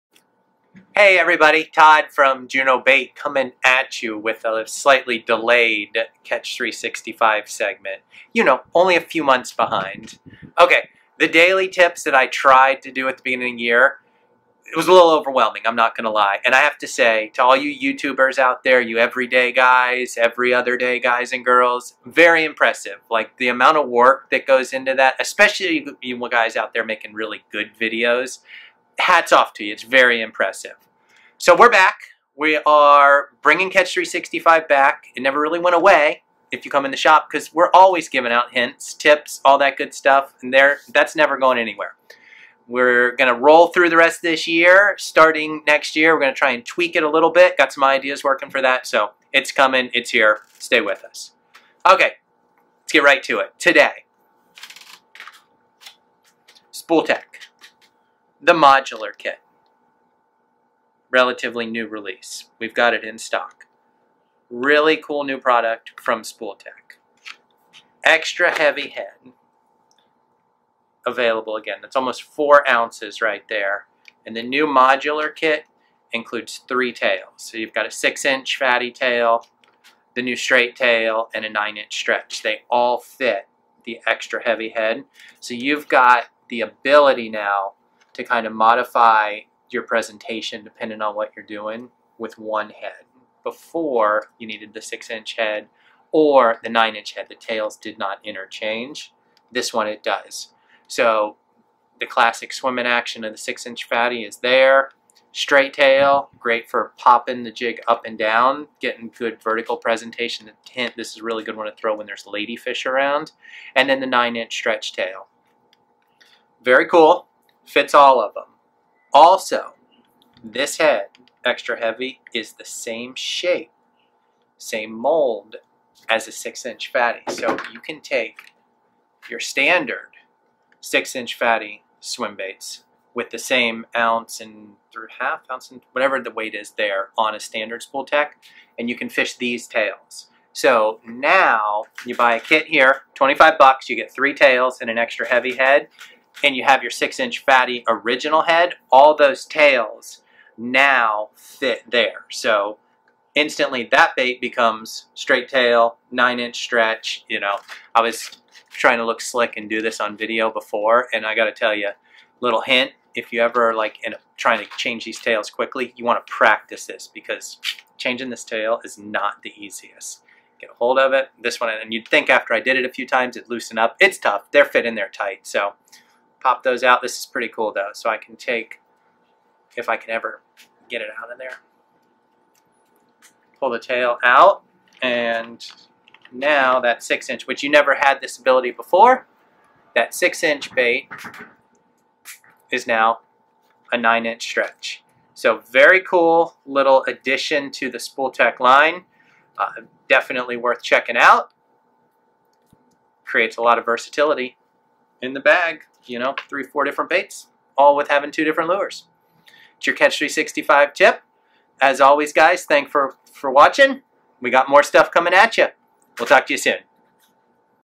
The Extra Heavy is BACK...and it's EXTRA Awesome!
SpoolTek lures re-released the "Extra Heavy" in a Modular kit...it's pretty awesome! Check it out...